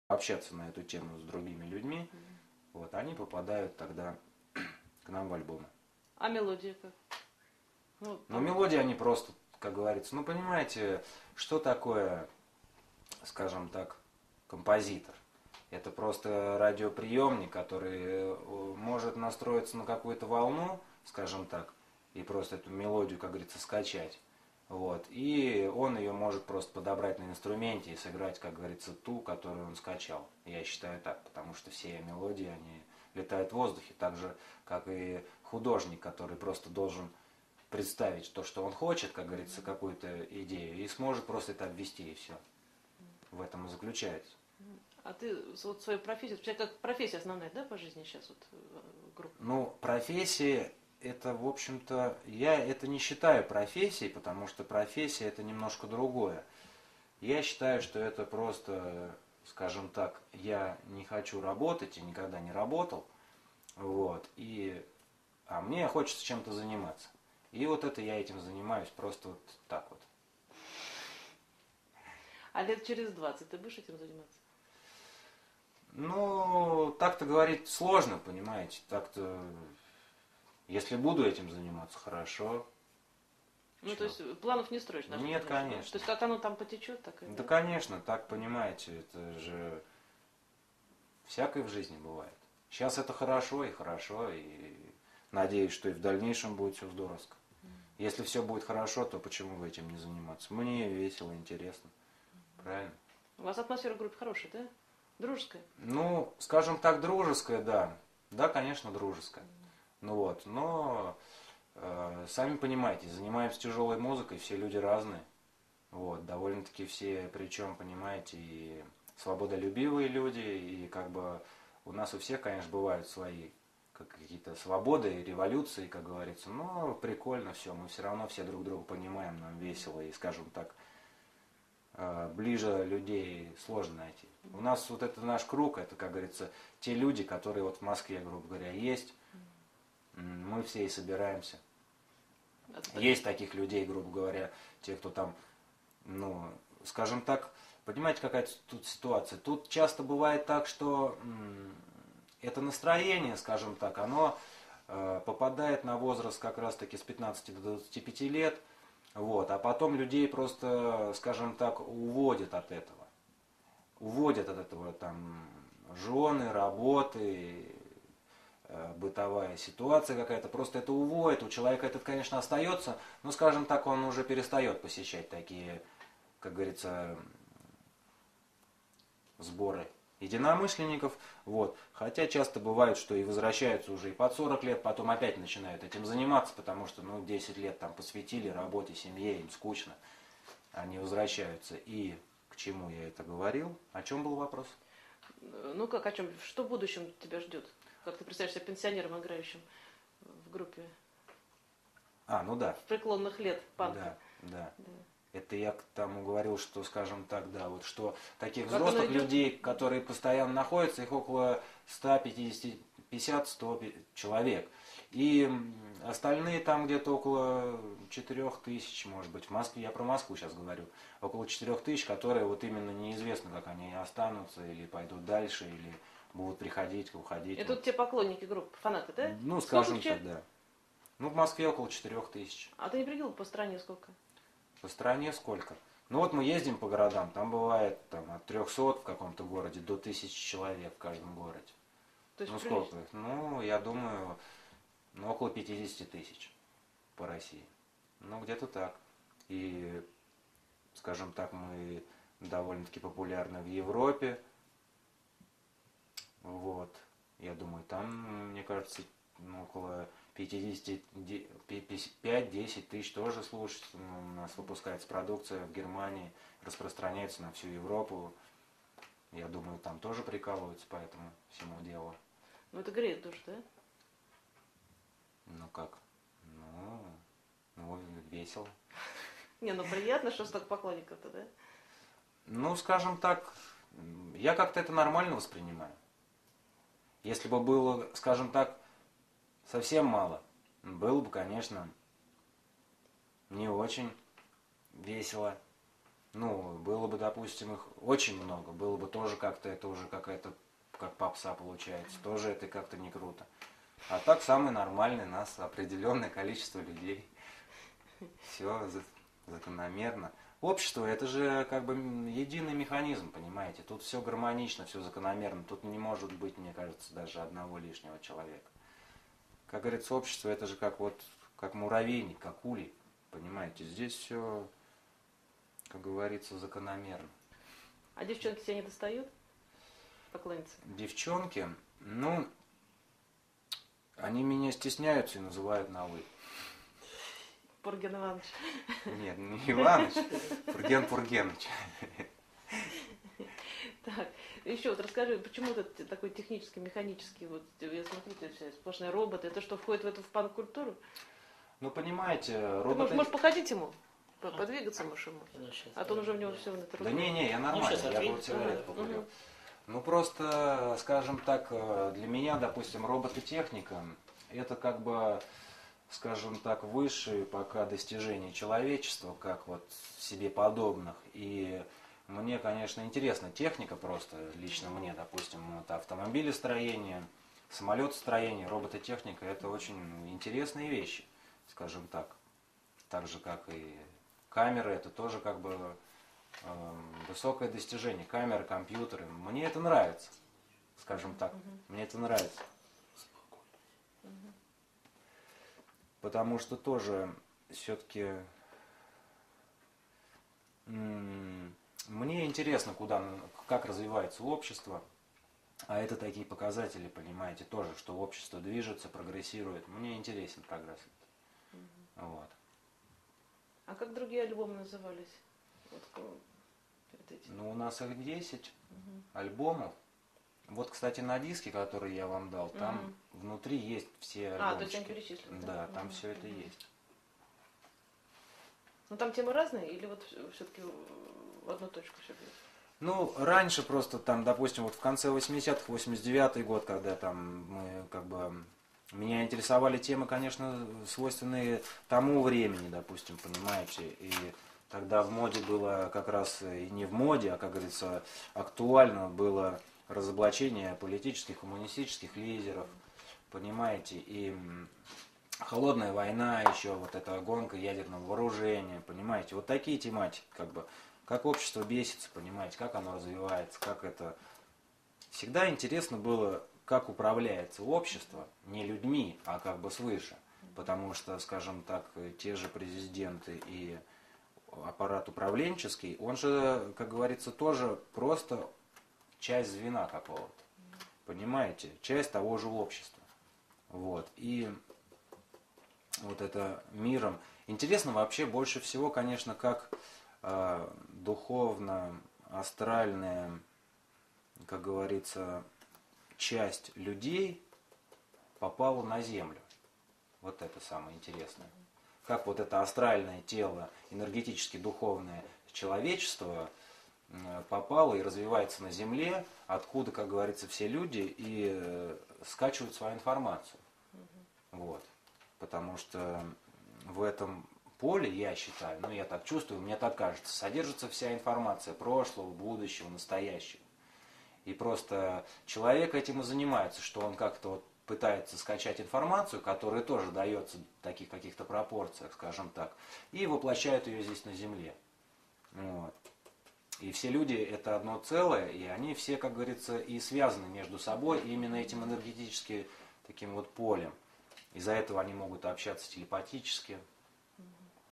Purgen Interview 2 (2/3)
Weird Purgen interview found on purgen.3dn.ru. Not sure about the origin of this, but it seems, that it was some dvd release in 2007.